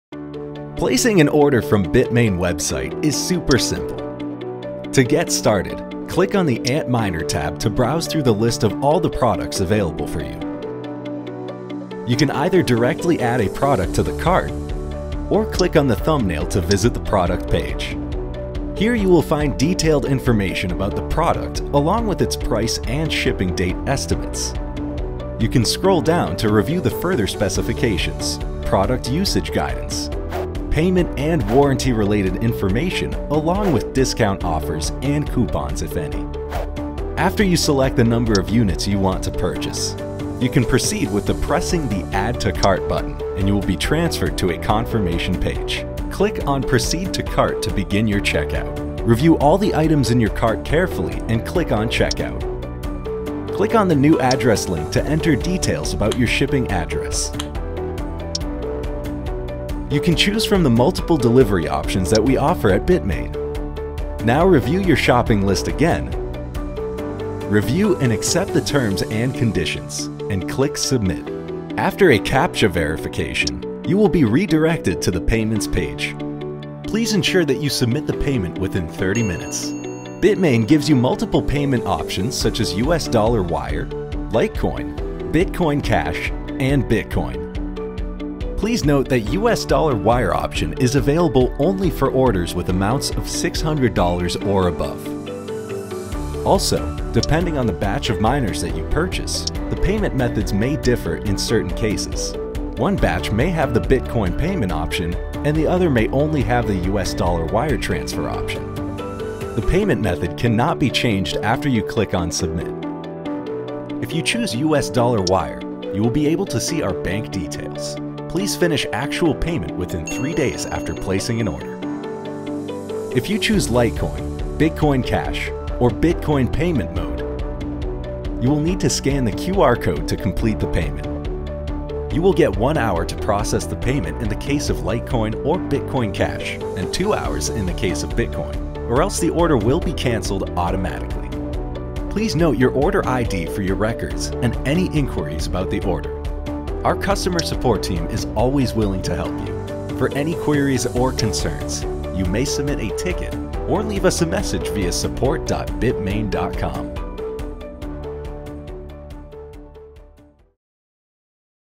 How to Make Purchase on the Bitmain Website
A quick and easy guide on how to successfully purchase products on the Bitmain website.